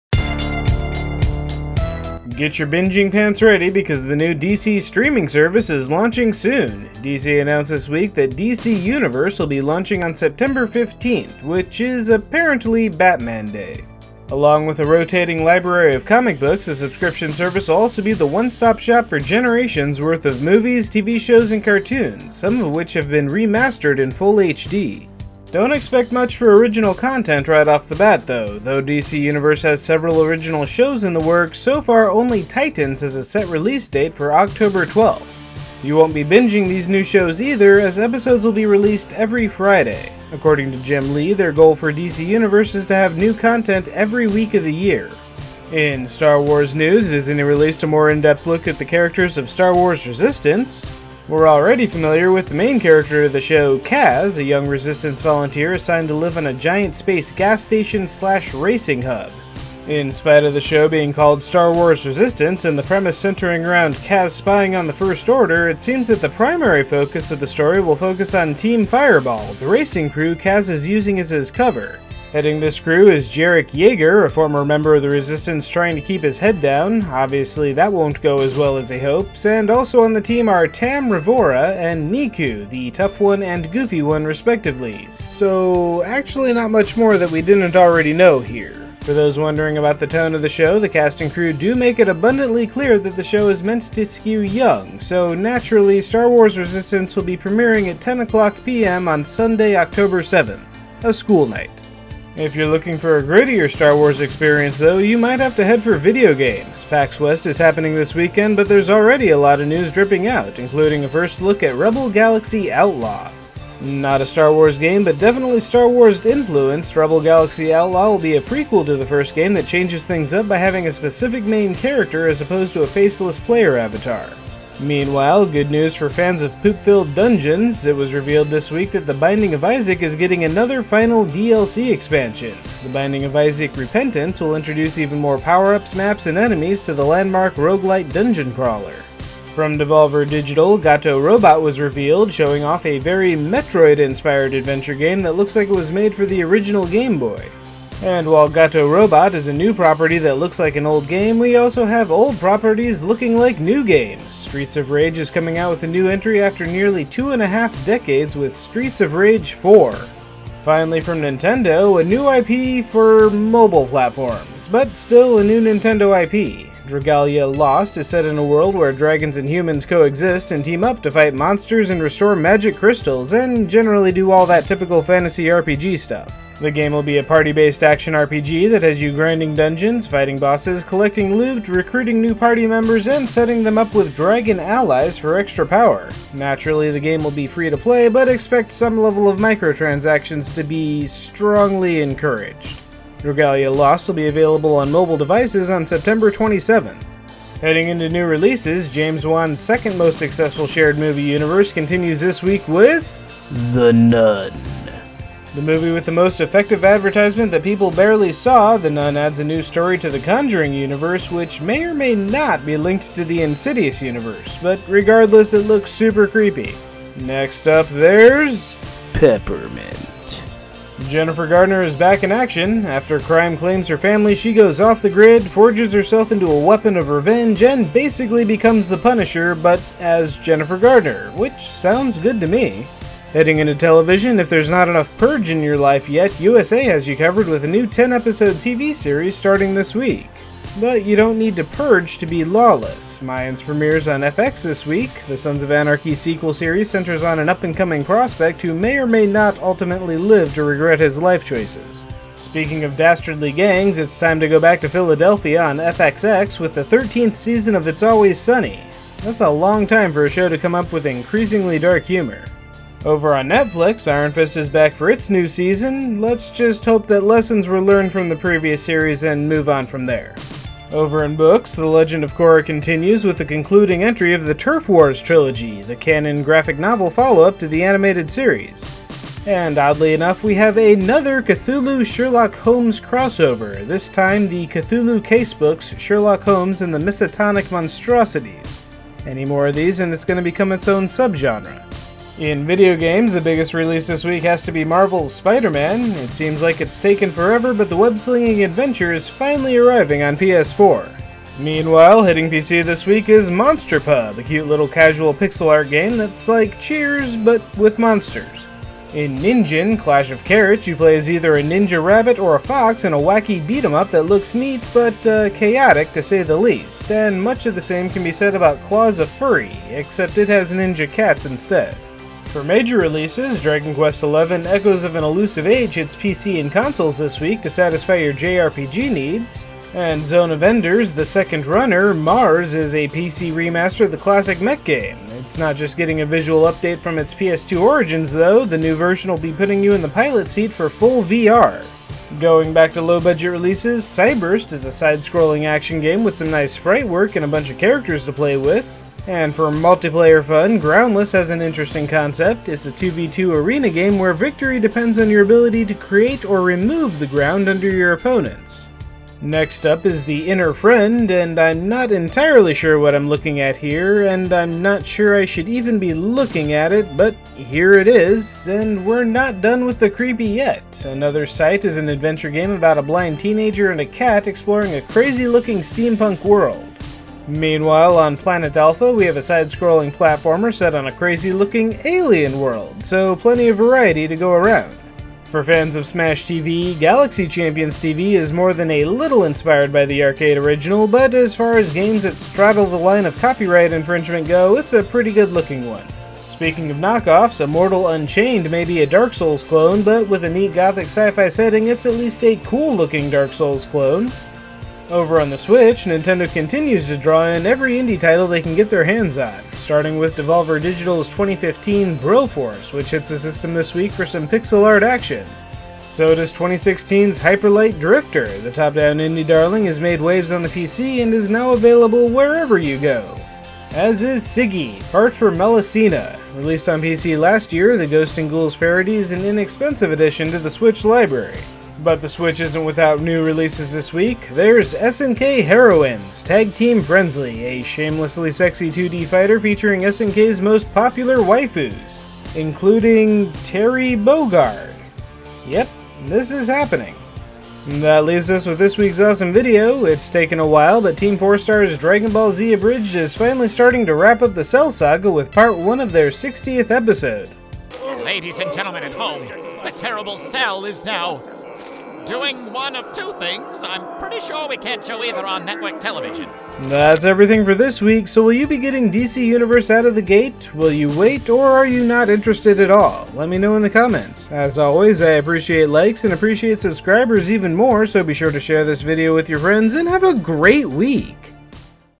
TWN: DC UNIVERSE TO LAUNCH ON BATMAN DAY
As tempting as Batman: TAS in HD is, I think I'll probably wait until next year when Young Justice comes out.